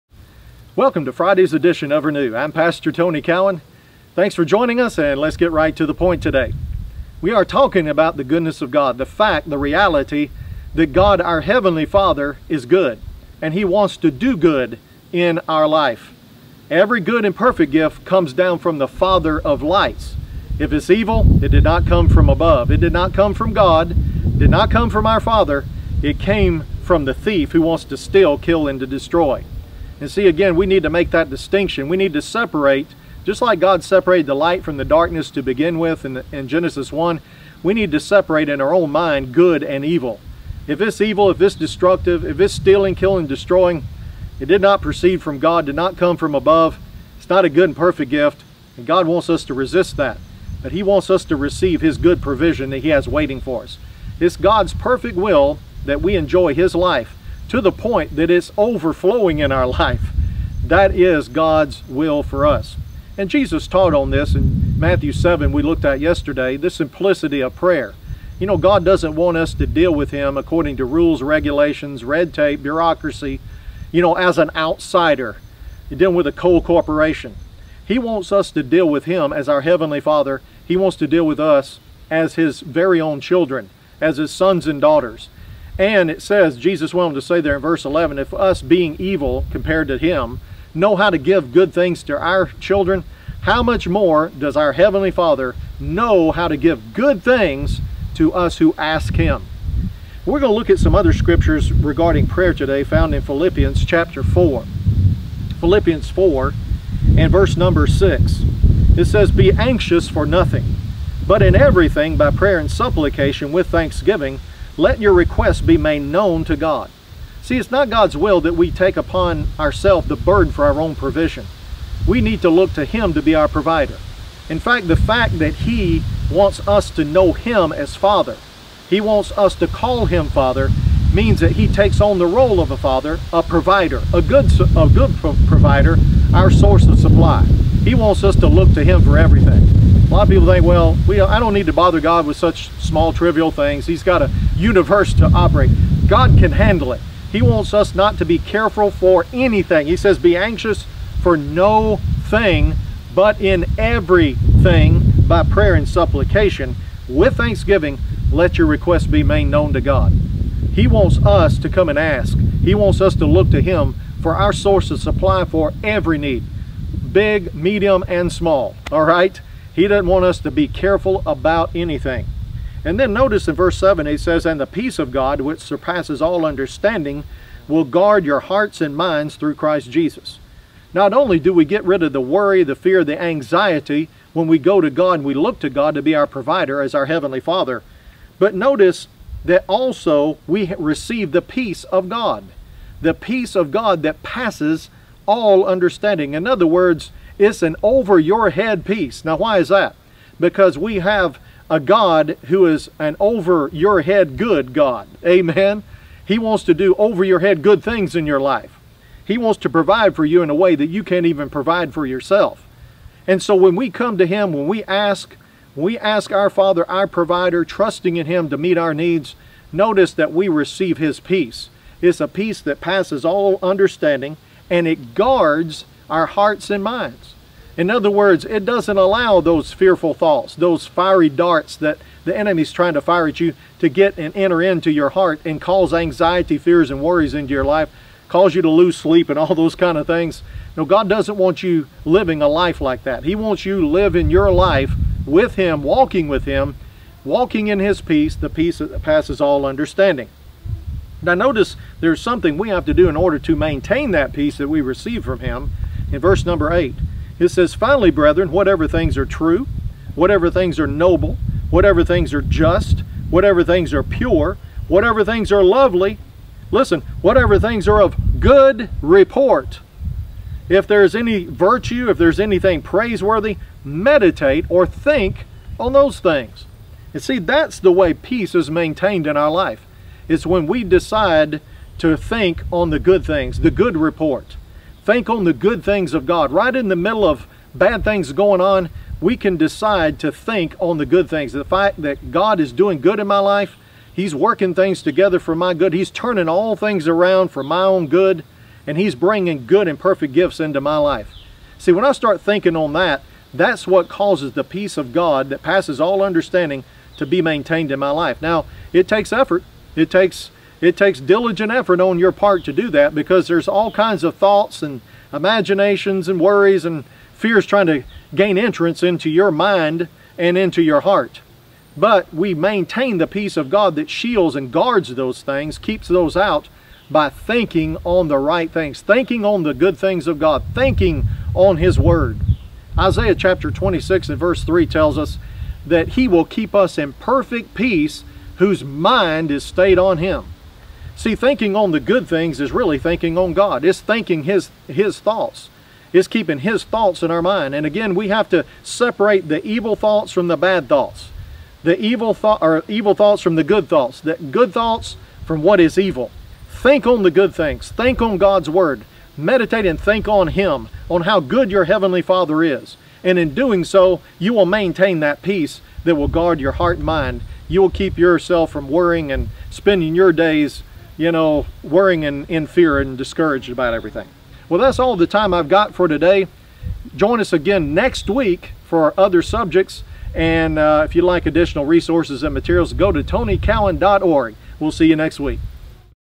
Renew (episode 50)
God is Good!:   Vol. 1, Week 2, Day 5

This bite-sized daily podcast is designed to help you grow in your daily walk with God.  This is week 2 of the very important subject of God's goodness - the reality that He is good and does only good.  Day by day we are discovering the truths that will help renew our minds to the new life that we have in Jesus.  Welcome to Friday's episode!